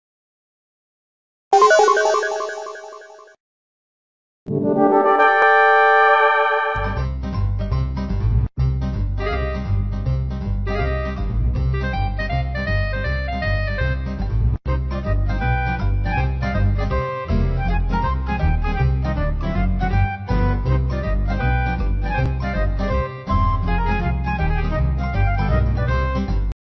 Chesnaught Animation
Animation displaying the dancing power of Chesnaught

Animated using FlipaClip